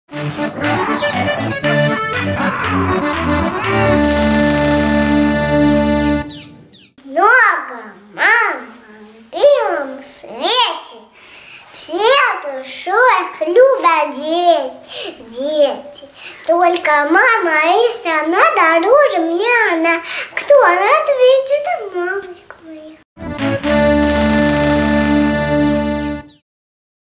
Анюта Радкевич - тема "Моя мама" | Детский проект "Стих на табуретке"
3 года